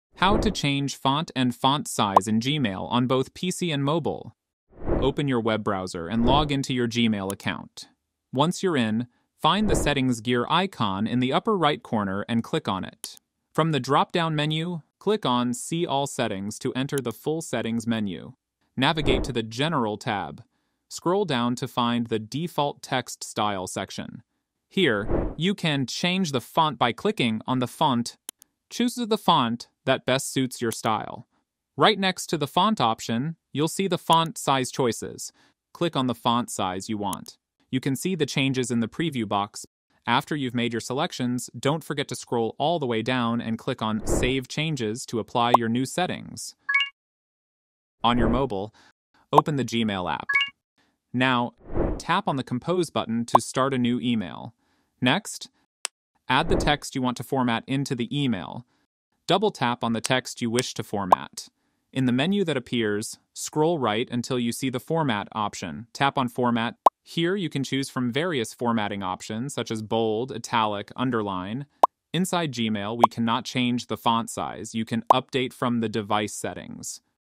How to Change Font & Font Size in Gmail on both PC & Mobile?
Learn how to easily change the font and font size in Gmail on both your PC and mobile device with this step-by-step tutorial. Whether you prefer a different font style or need to adjust the text size for better readability, this guide will show you how to personalize your Gmail experience. Watch now to customize your Gmail fonts to suit your preferences!